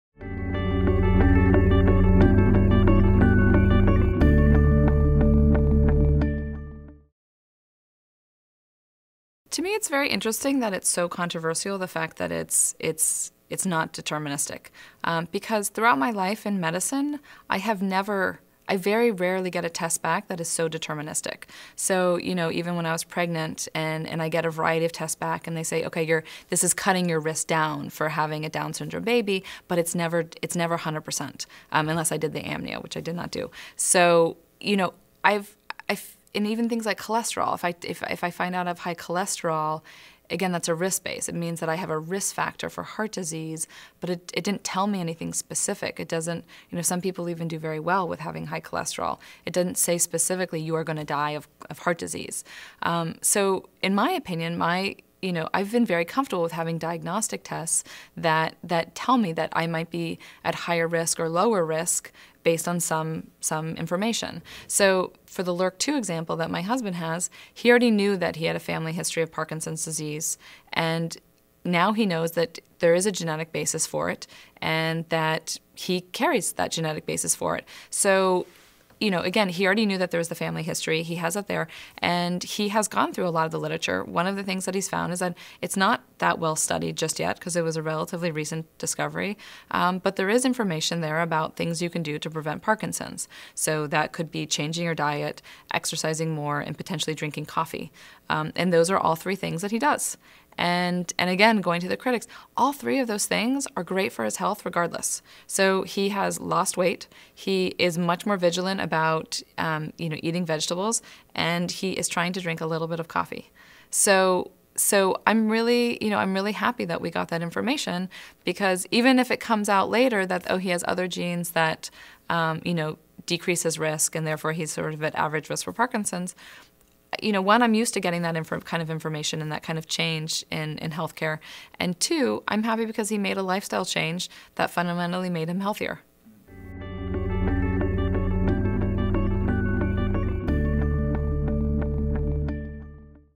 A Test that Changed Sergey Brin's Life | Anne Wojcicki | Big Think
A Test that Changed Sergey Brin's Life
For the founder of Google, genetic testing uncovered a serious risk factor. His wife Anne Wojcicki, the co-founder of 23andMe, describes what happened.
Anne Wojcicki:

Anne's background is in healthcare investing, focused primarily on biotechnology companies. Anne left the investing world with the hope that she could have a positive impact on research and medicine through 23andMe. From her vantage point, Anne saw a need for creating a way to generate more information - especially more personalized information - so that commercial and academic researchers could better understand and develop new drugs and diagnostics. By encouraging individuals to access and learn about their own genetic information, 23andMe will create a common, standardized resource that has the potential to accelerate drug discovery and bring personalized medicine to the public. Anne graduated from Yale University with a B.S. in biology.
TRANSCRIPT:

Question: Describe how genetic testing has affected your life.

Anne Wojcicki: To me it’s very interesting that it’s so controversial the fact that is it not deterministic. Because throughout my life in medicine, I very rarely get a test back that is so deterministic. So, even when I was pregnant and I get a variety of tests back and they say, okay this is cutting your risk down for having a Down Syndrome baby, but it’s never 100 percent, unless I did the amnio, which I did not do. And even things like cholesterol, if I find out I have high cholesterol, again it’s a risk base. It means I have a risk factor for heart disease, but it didn’t tell me anything specific. Some people even do very well with having high cholesterol. It doesn’t say specifically you are going to die of heart disease. So, in my opinion, I’ve been very comfortable with having diagnostic tests that tell me that I might be at higher risk, or lower risk based on some information. So for the LRRK2 example that my husband has, he already knew that he had a family history of Parkinson’s disease and now he knows that there is a genetic basis for it and that he carries that genetic basis for it. He already knew that there was the family history, he has that there, and he has gone through a lot of the literature. One of the things that he’s found is that it’s not that well studied just yet because there was a relatively recent discovery, but there is information there about things you can do to prevent Parkinson’s. So, that could be changing your diet, exercising more, and potentially drinking coffee. And those are all three things that he does.

And again, going to the critics. All three of those things are great for his health regardless. So, he has lost weight, he is much more vigilant about eating vegetables and he is trying to drink a little bit of coffee. So, I’m really happy that we got that information because even if it comes out later that, oh, he has other genes that decrease his risk, and therefore he’s sort of an average risk for Parkinson’s; one, I am used to getting that kind of information and that kind of change in healthcare, and two, I’m happy because he made a lifestyle change that fundamentally made him healthier.